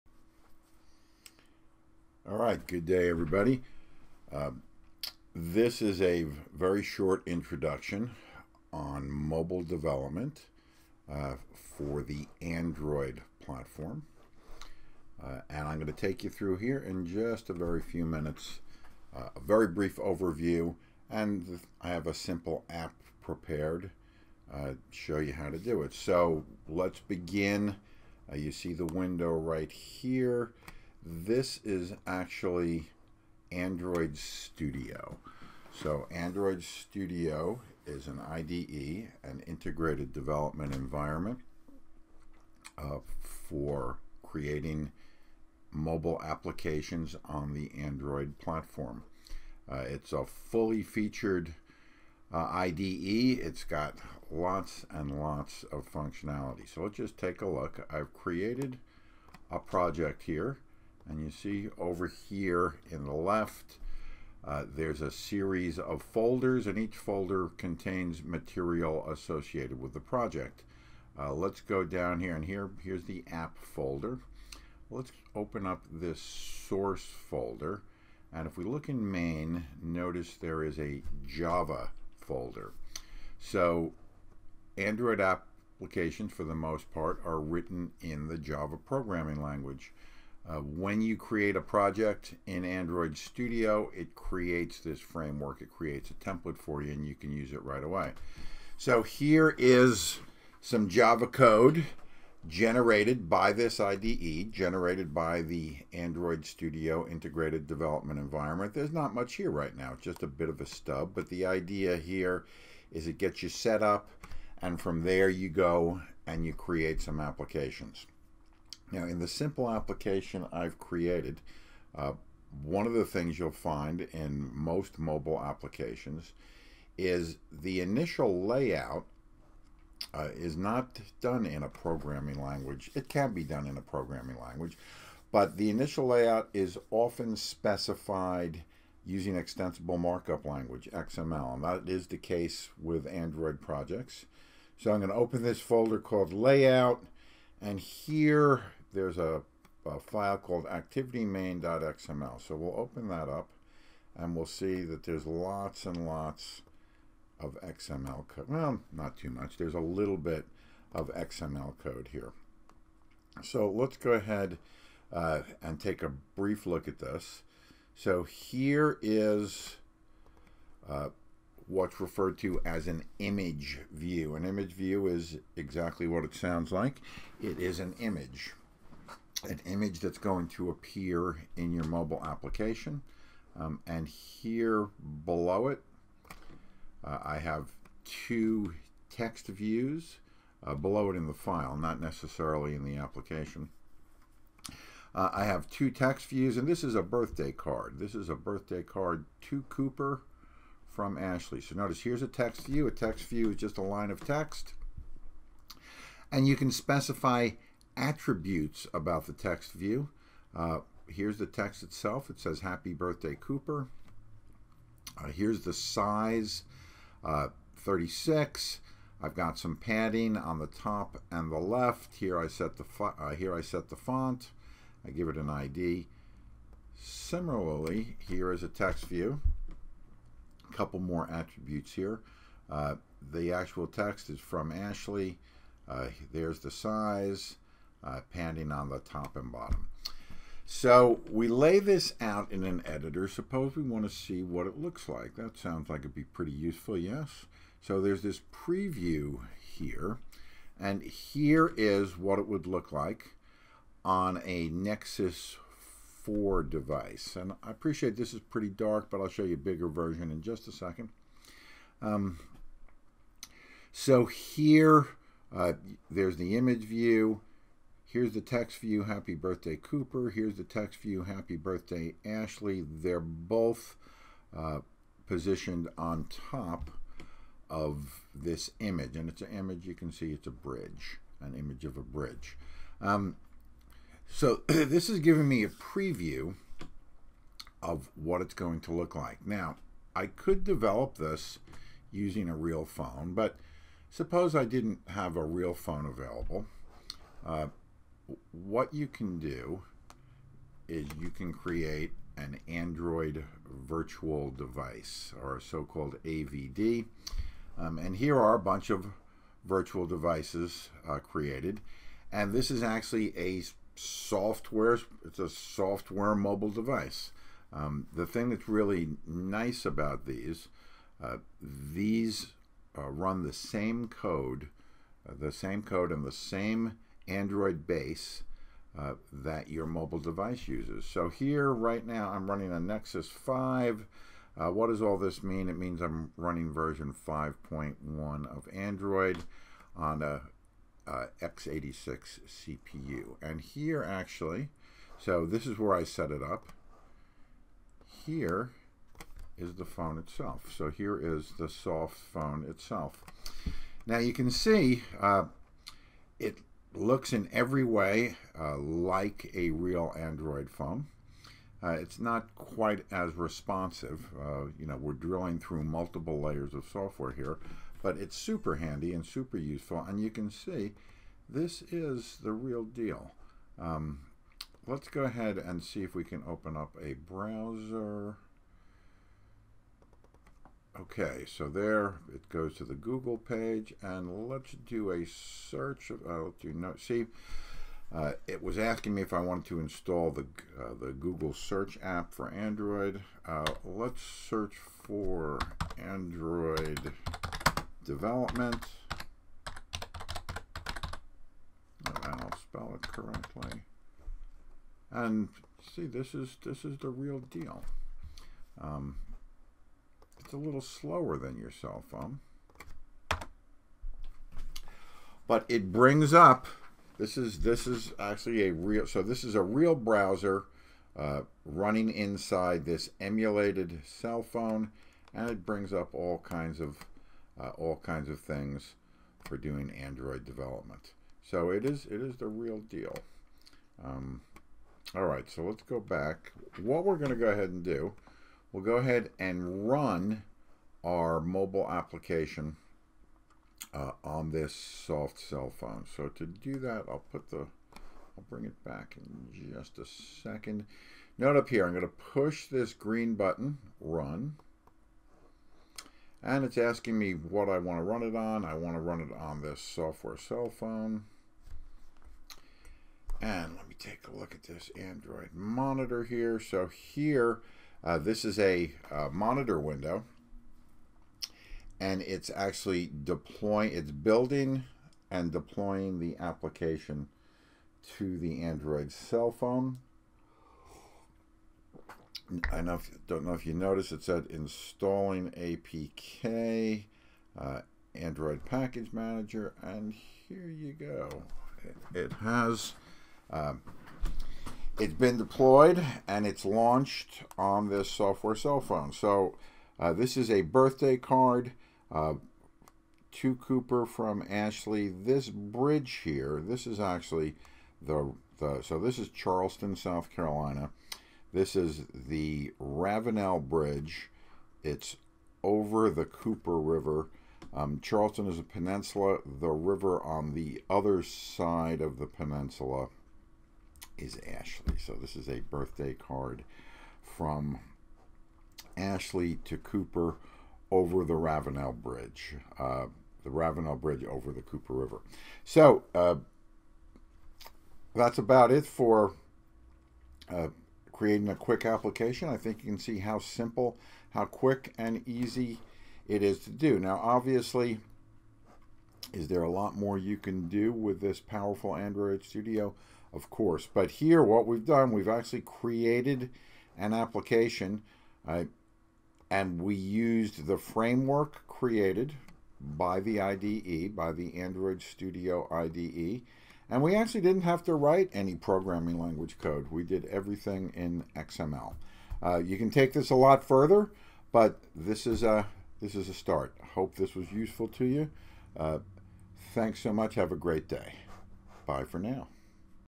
Developing a Simple Mobile Application with Android Studio
Using Android Studio to develop a simple mobile application.